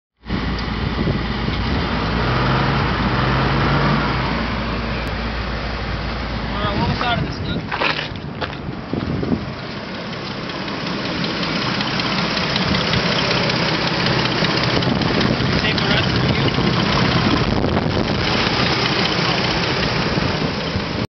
GovDeals: 2002 Oldsmobile Bravada AWD
2002 Oldsmobile Bravada AWD sold by West Tennessee Violent Crime and Drug Task Force, TN on GovDeals.com